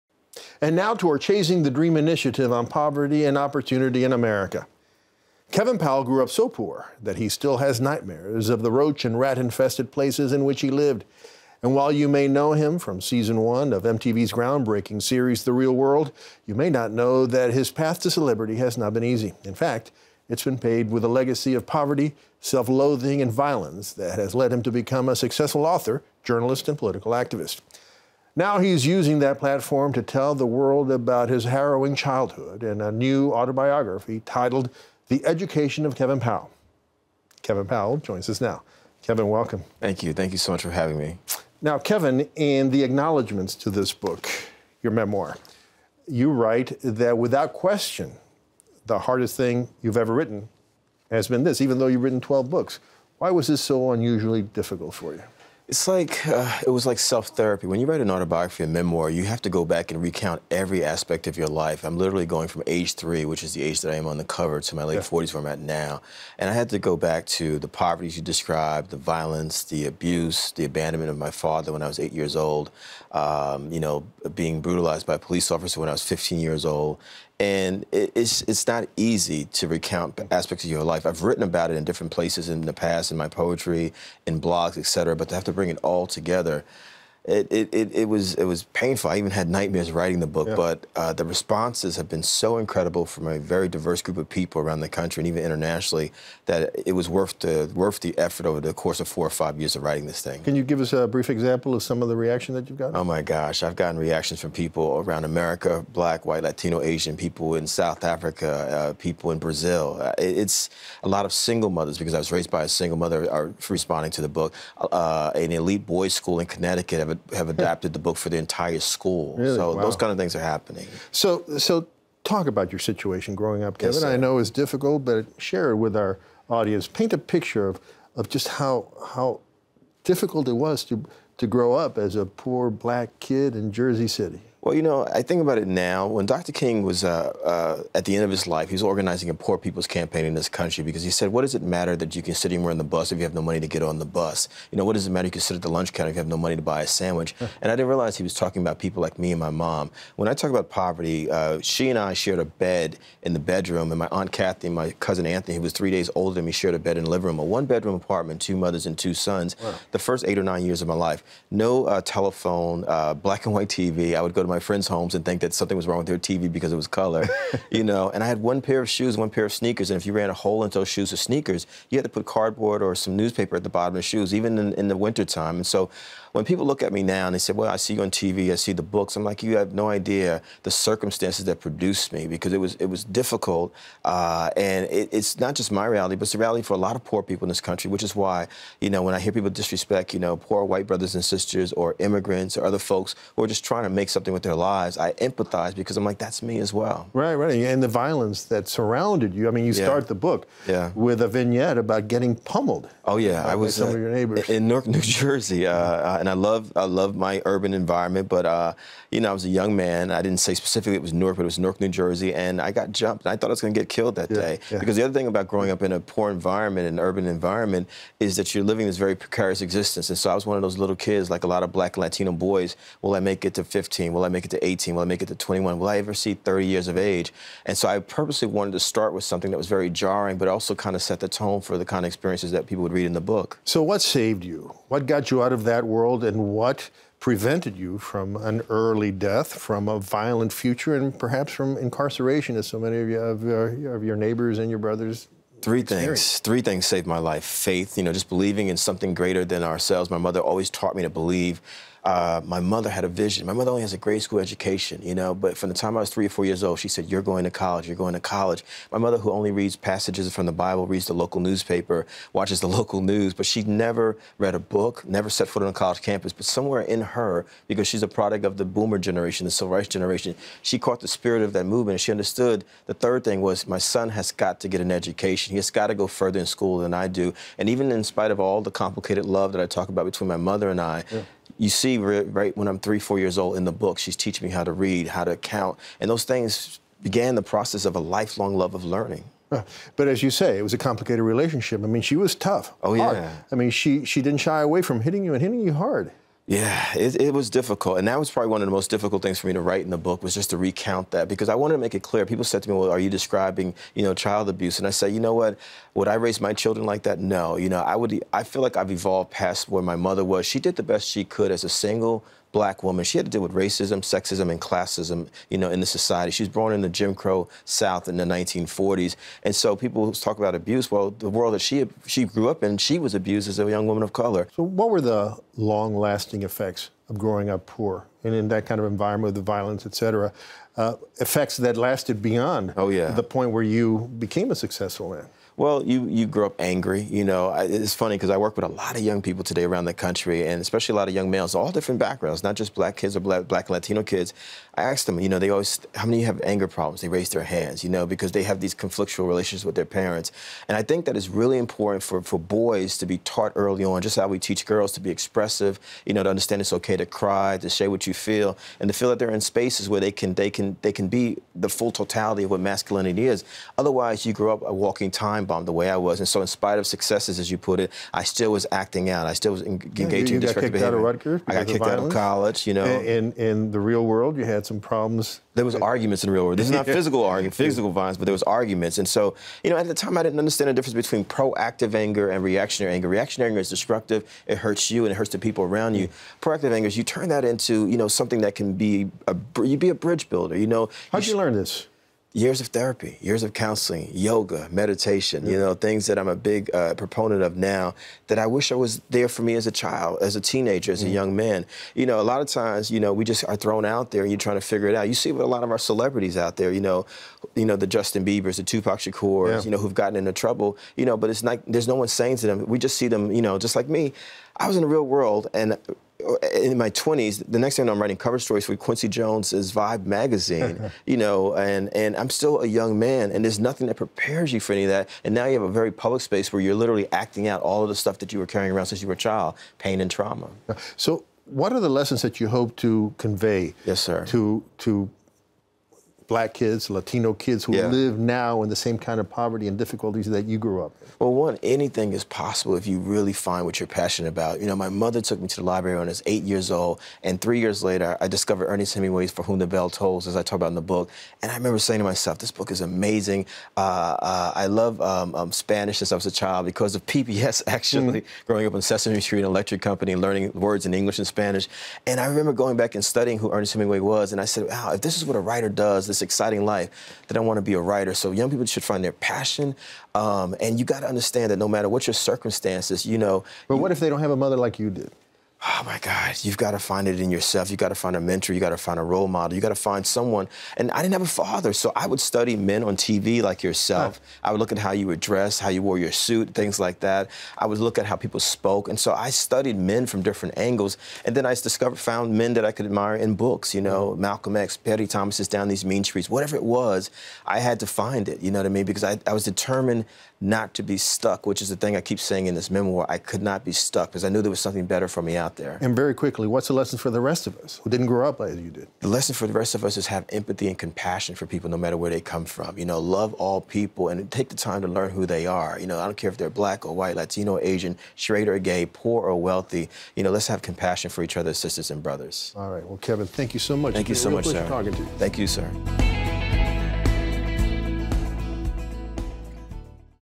How Kevin Powell Overcame Poverty
From poverty and abuse, to reality TV fame, to published author,  Kevin Powell, a former star of MTV’s “Real World,” shares his personal story about chasing and achieving his dreams.